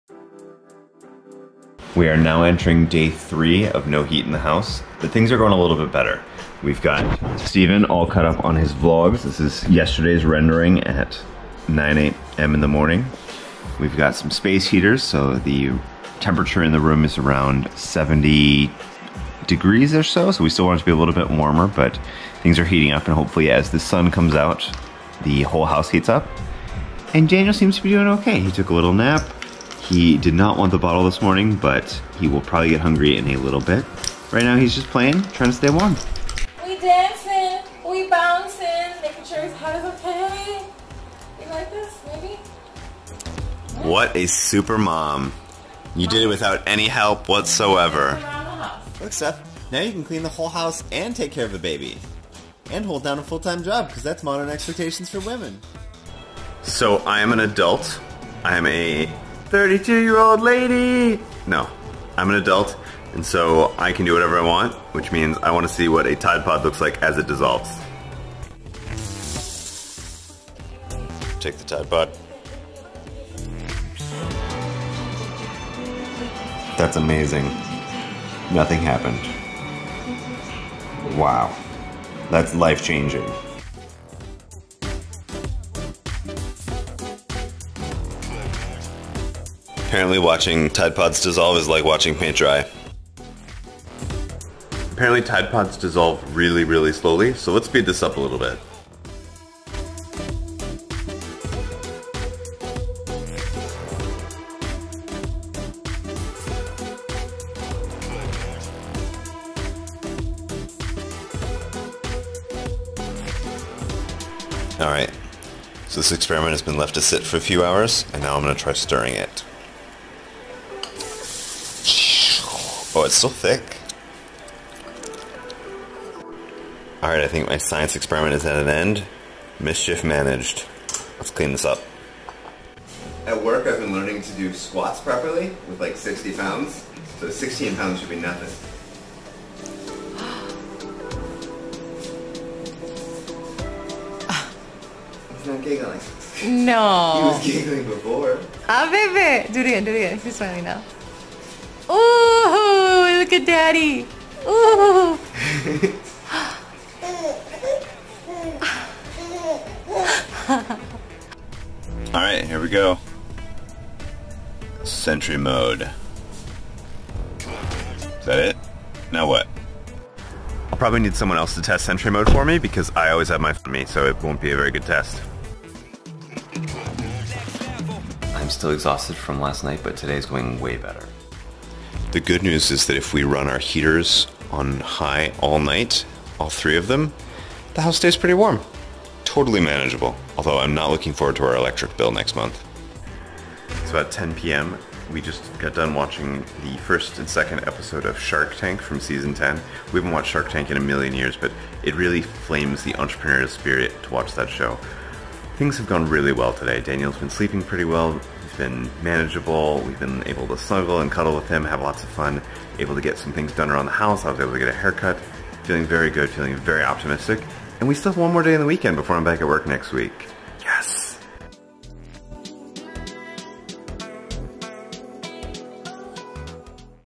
what does a Tide pod look like as it dissolves? - Ep 2.050
I take today to be a bit of a childlike experimenter. Looking at modern things like Tide pods makes me wonder what happens to it in the washing machine.